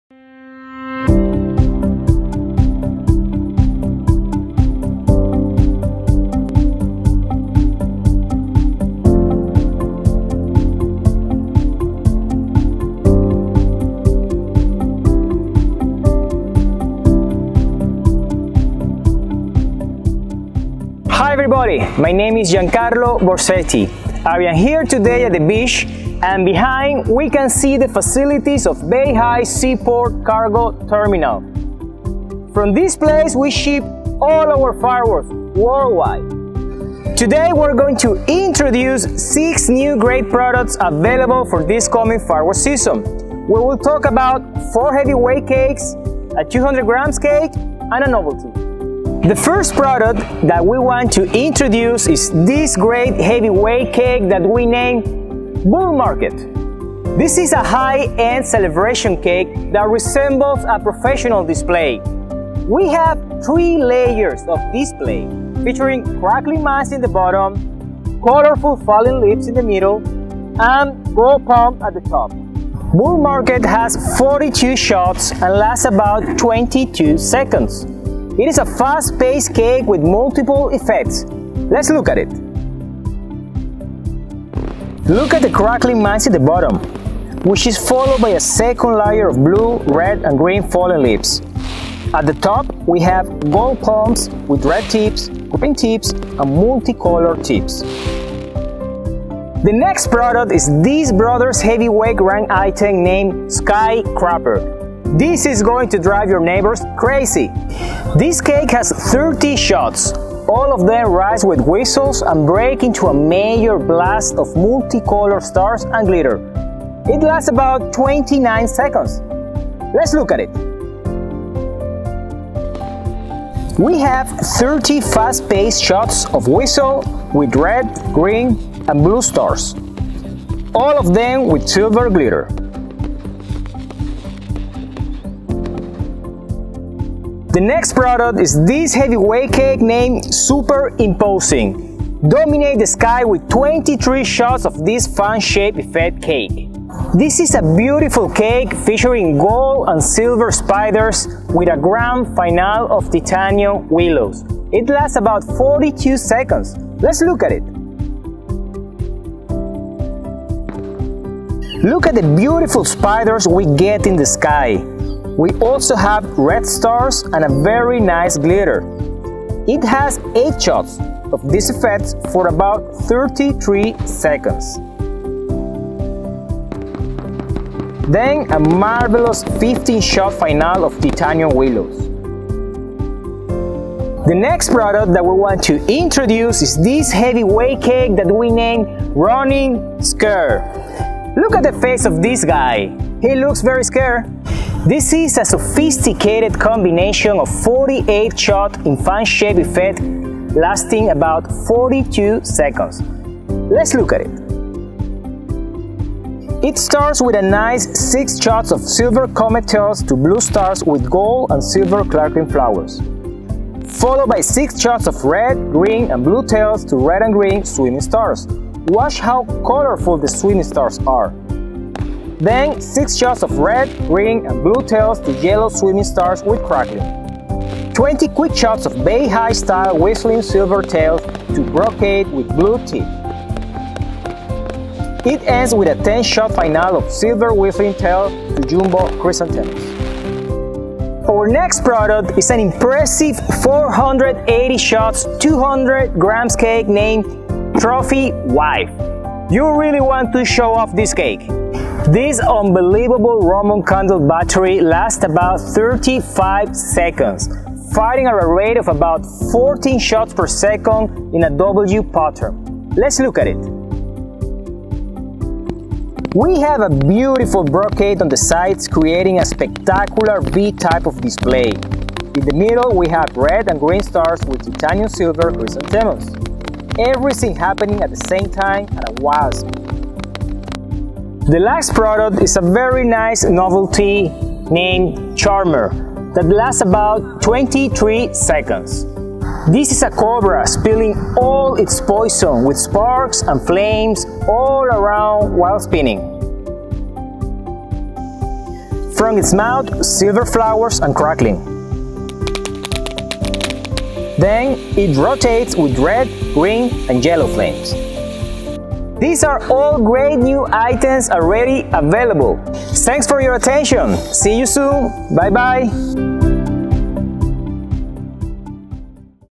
Brothers 2021 New Products Presentation #7
From the facilities of Beihai Sea Port Cargo Terminal we walk through the main features and present a shorter video version of the products performance describing the fireworks effects and effects sequences of: